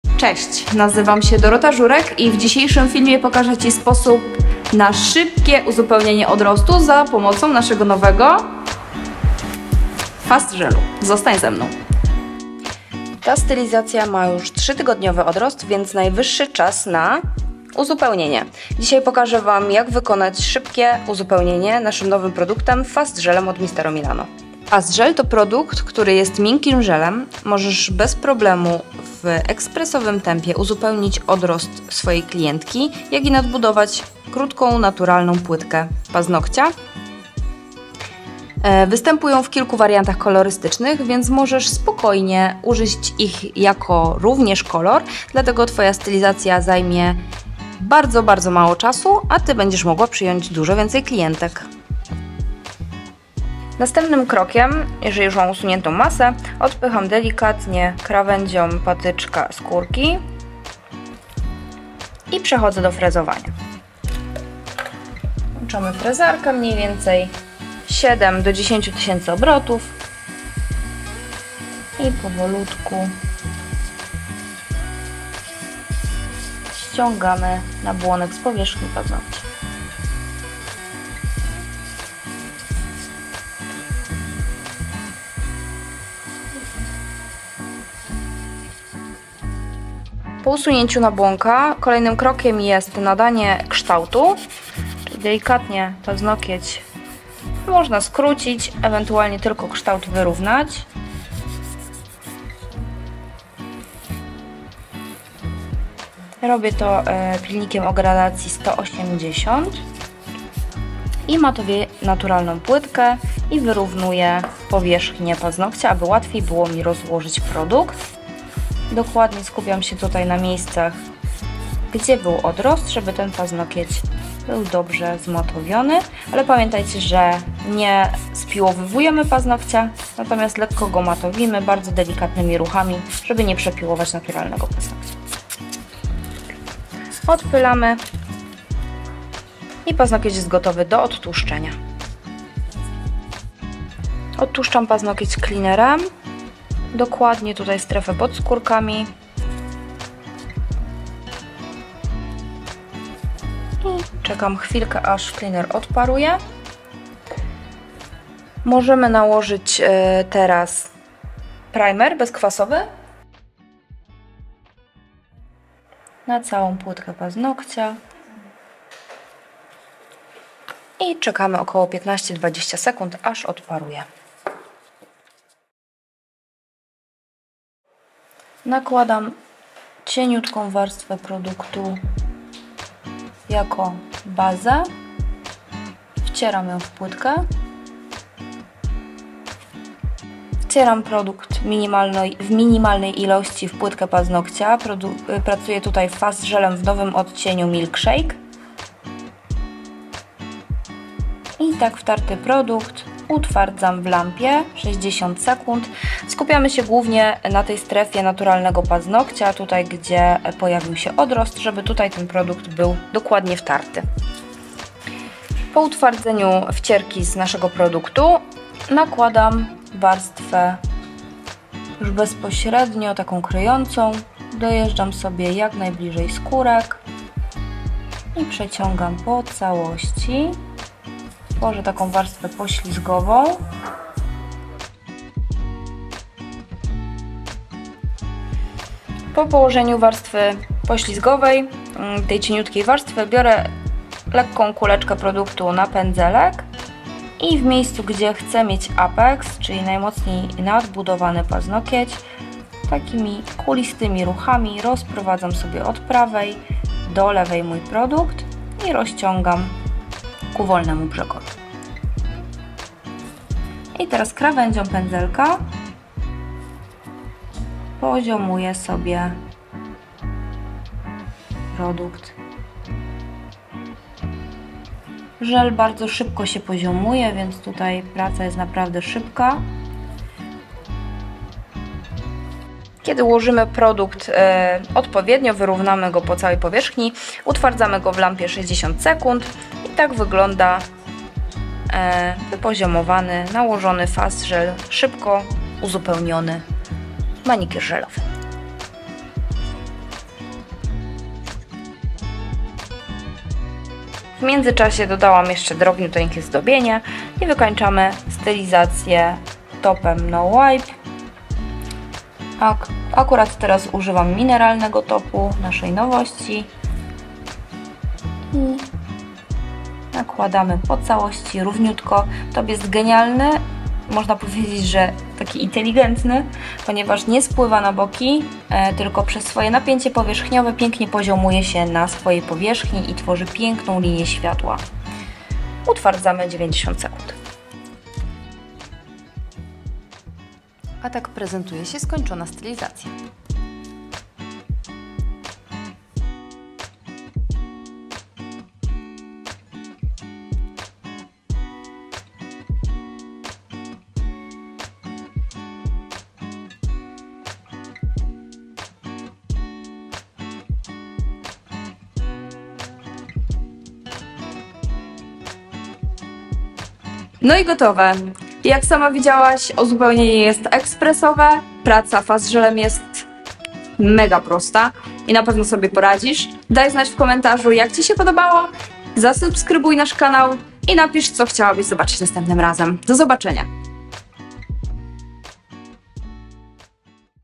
💅UZUPEŁNIENIE ODROSTU FAST GELEM | COMPLEMENTING THE GROWTH WITH FAST GEL | MISTERO MILANO💅
Odkryj moc żelu w pędzelku i zobacz jak łatwo i szybko możesz wykonać nim ekspresowe uzupełnienie paznokci!
Fast Gel to produkt, który jest miękkim żelem, dzięki któremu możesz bez problemu, w ekspresowym tempie uzupełnić odrost swojej klientki jak i nadbudować krotka naturalna płytkę paznokcia.
Fast Gel występuje w kilku wariantach kolorystycznych, można użyć go również jako koloru bazowego tworząc ekspresowe stylizacje.

Discover the power of the gel in the brush and see how easy and fast you can make an express nail filling with it!
Fast Gel is a product that is a soft gel, thanks to which you can easily replenish your client's regrowth and build up a short natural nail plate.
Fast Gel is available in several color variants, you can also use it as a base color for express styling.